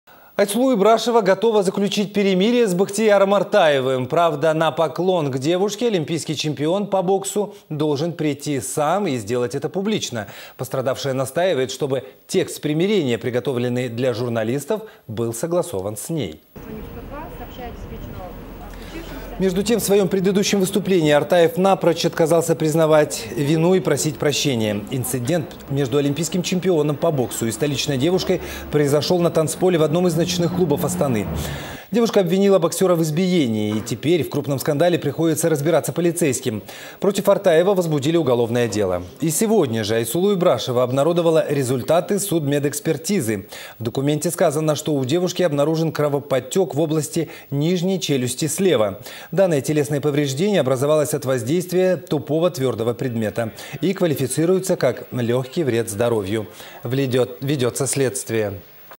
Айсулу Ибрашова готова заключить перемирие с Бахтияром Артаевым
Айсулу Ибрашова готова заключить перемирие с Бахтияром Артаевым. Правда, на поклон к девушке олимпийский чемпион по боксу должен прийти сам и сделать это публично. Пострадавшая настаивает, что бы текст примерения приготовленный для журналистов был согласован с ней.